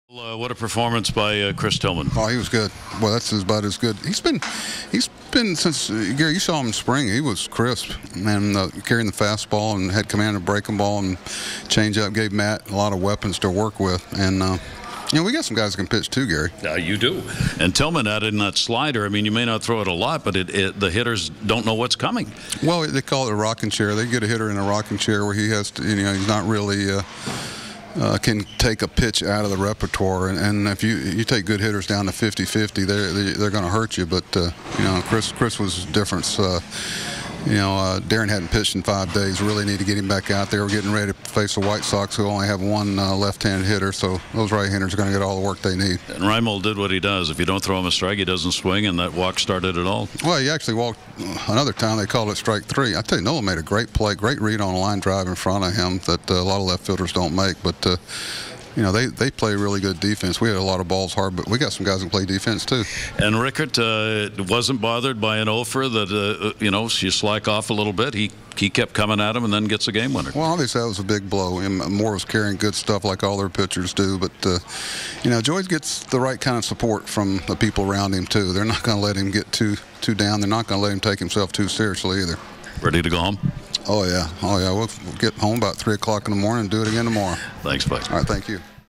Buck Showalter on the O's 3-1 win
Buck Showalter is now tied for 30th on the all-time managerial wins list with 1,352 career wins. More